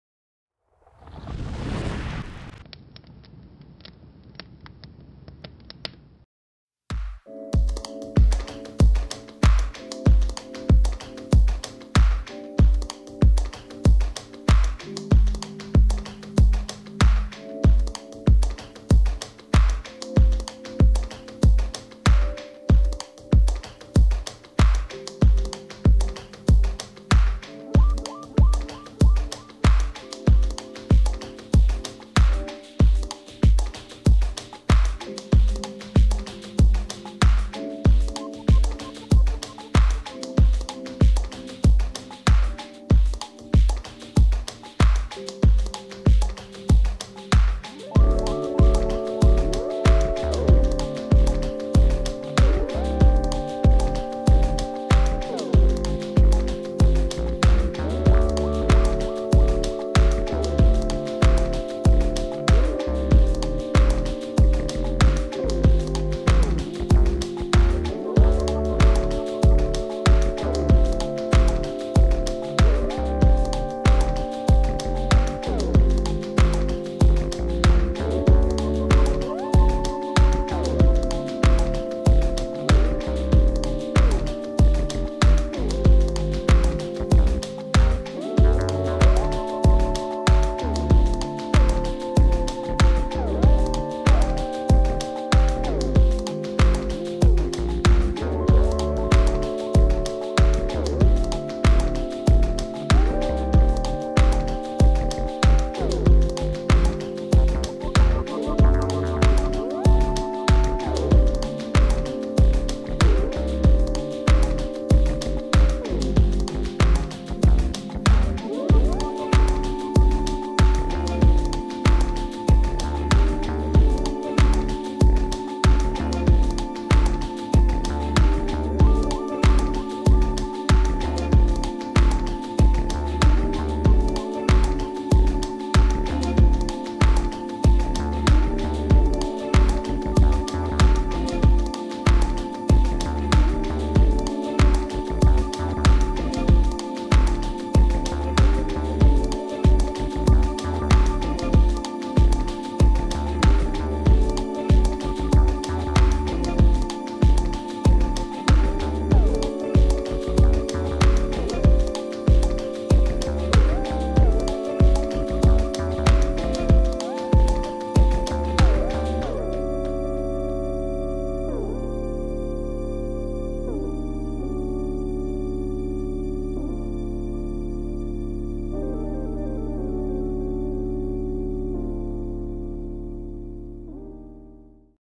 Timelapse Oil Painting: Cast Study
This is a 3 hour cast study I made in my free time and to train my observation skills.